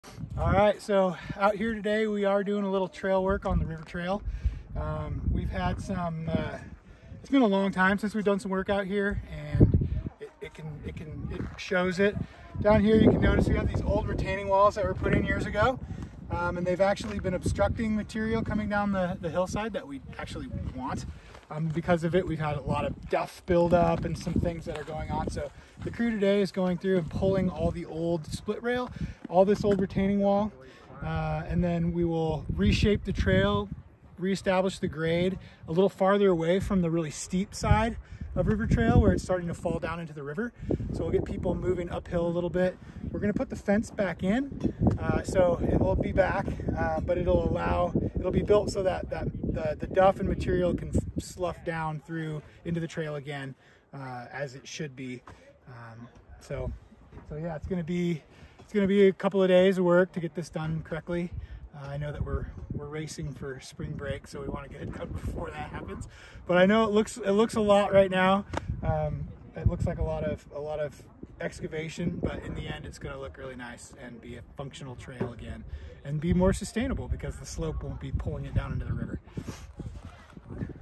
Springtime Trail Maintenance on the River Trail at Smith Rock
Smith Rock State Park Ranger Sam Vanderbeek is tearing up part of the River Trail with some volunteers from the Heart of Oregon Corps to do some much-needed repair work to save the trail from further erosion.